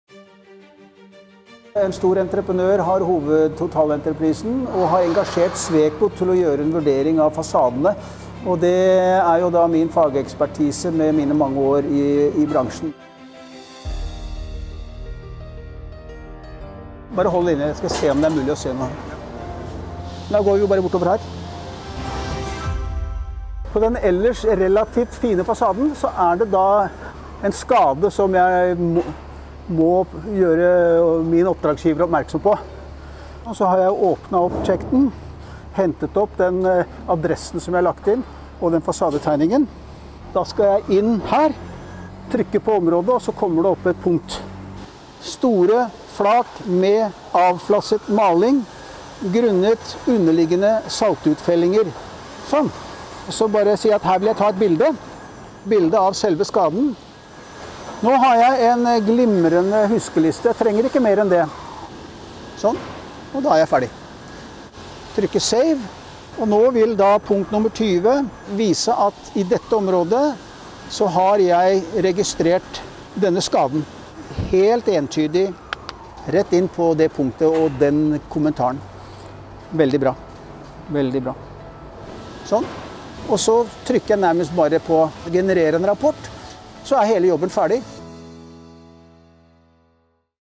Dokumenter Byggeprosjekt og Kvalitetssikring i Bygg og Anlegg | Effektivisering med Next
Rådgivende ingeniør J.C. Forester viser hvordan han bruker Next Field på sine befaringer.